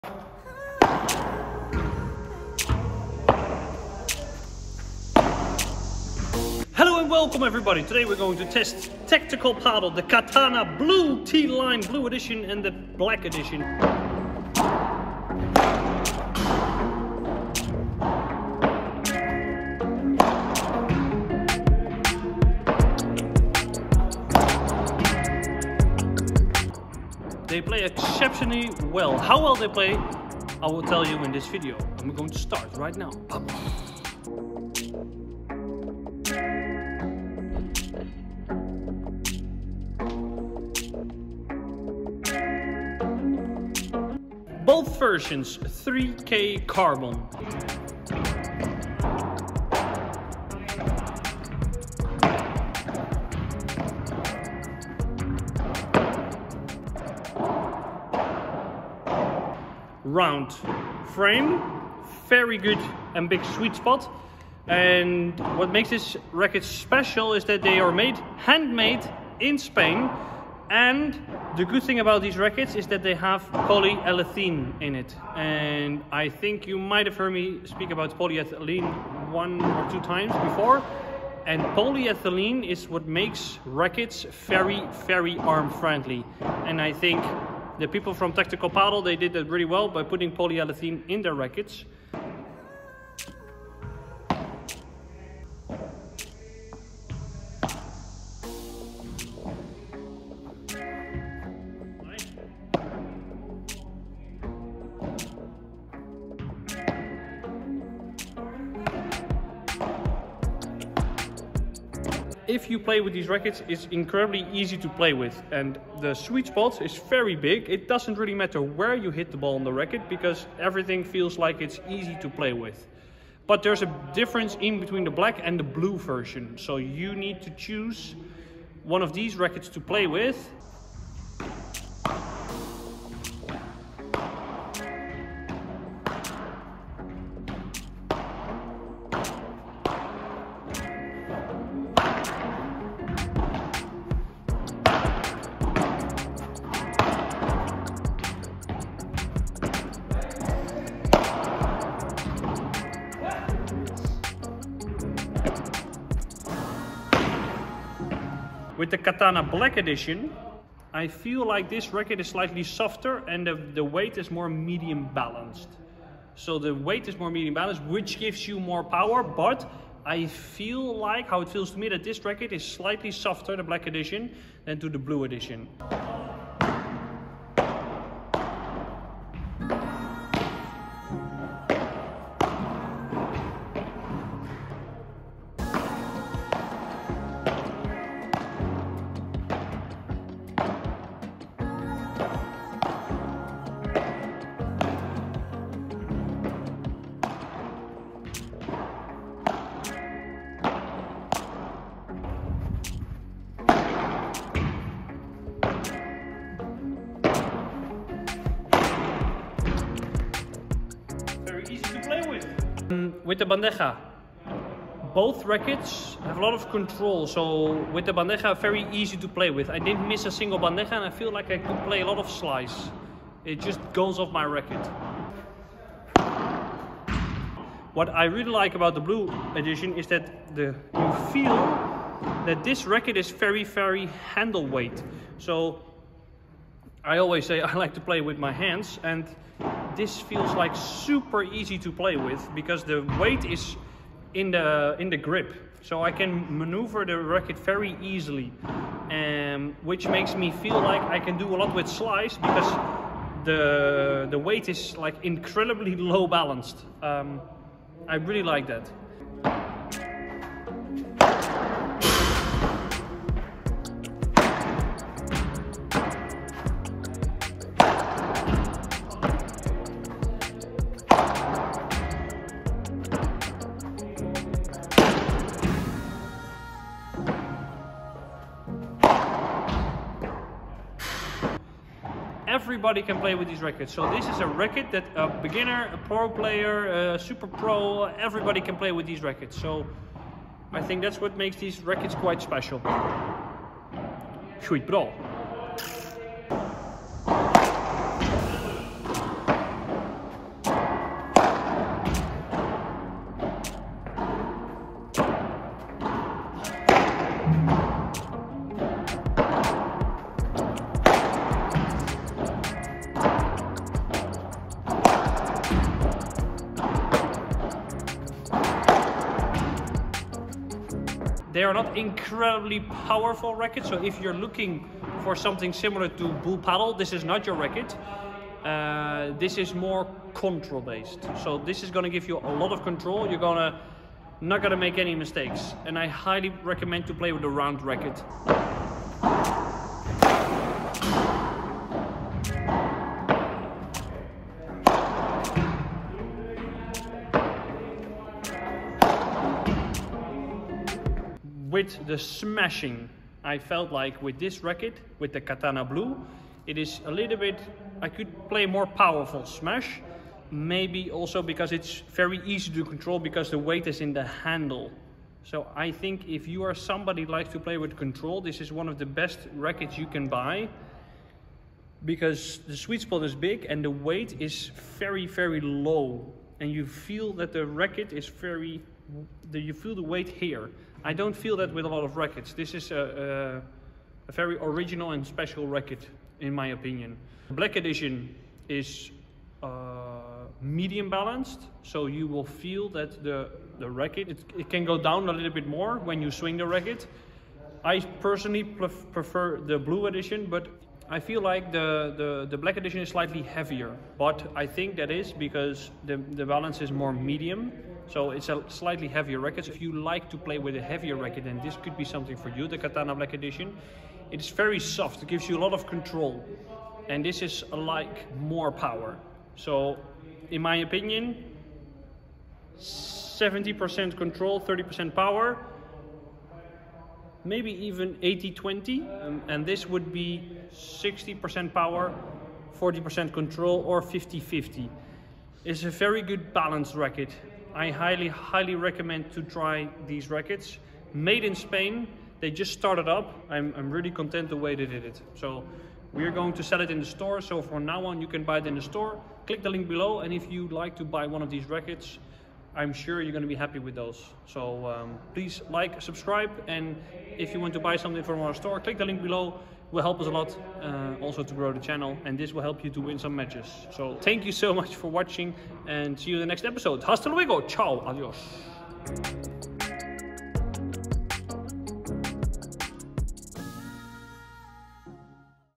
NEW Tactical Padel Rackets
These rackets are an hidden gem! Fantastic quality! If you like control and are looking to play out your opponents with precise shots, than this could be something for you!

Would you like to learn more? Become a Otro Nivel channel member or special member, support us and become better!